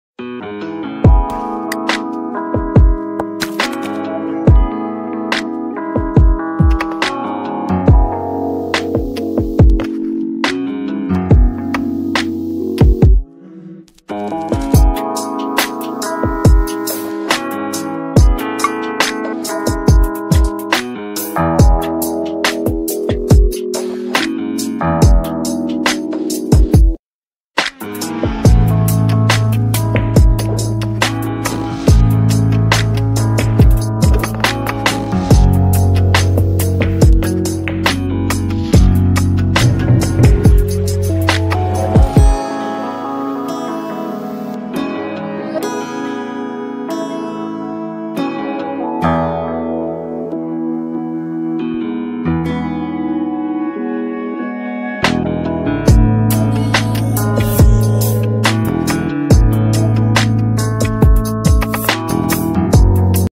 Best basic paper airplane tutorial
The easiest way to make a standard paper airplane! 👍